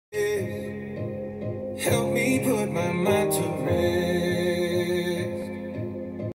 Weighted Sumo Squat
Proper Form:
1. Stand with feet wide and toes turned out.
2. Keep your chest up and abs tight
3. Try to squat at least 90 degrees or at least until the dumbbell touches the ground.
4. Push back up through your heels and think about pushing back up using your booty.